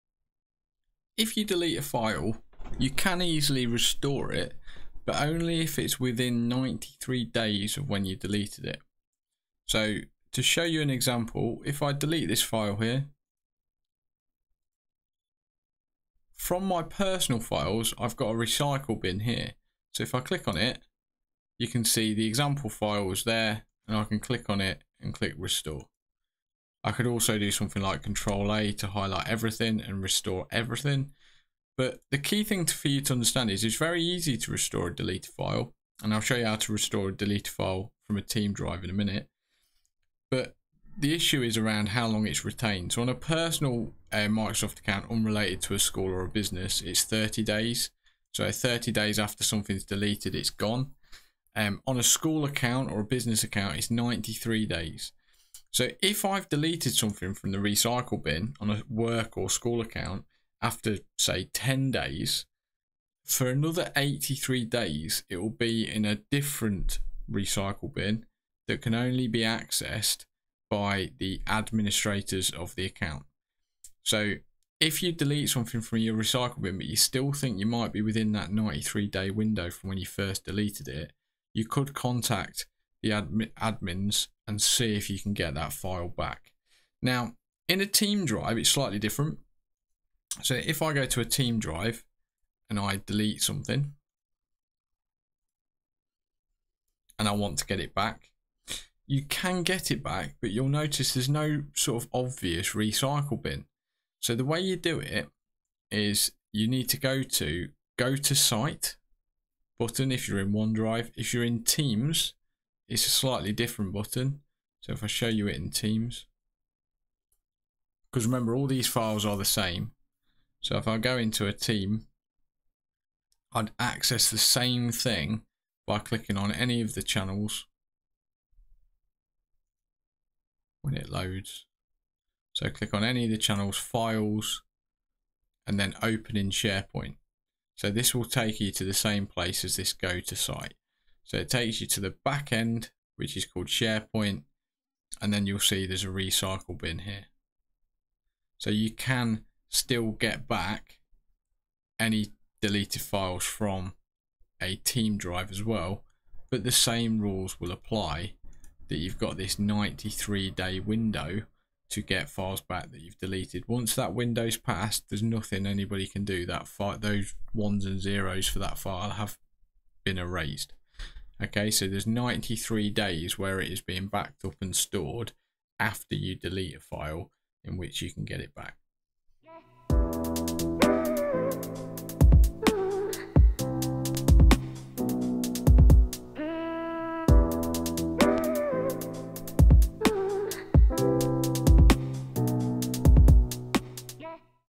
How To Restore A Deleted File On OneDrive or Teams
00:00 - Start
00:01 - How long Microsoft save deleted files for
00:09 - Example of deleting and restoring a file from OneDrive
00:40 - Can you recover a file deleted from OneDrive recycle bin
01:29 - Example of deleting and restoring a file from Teams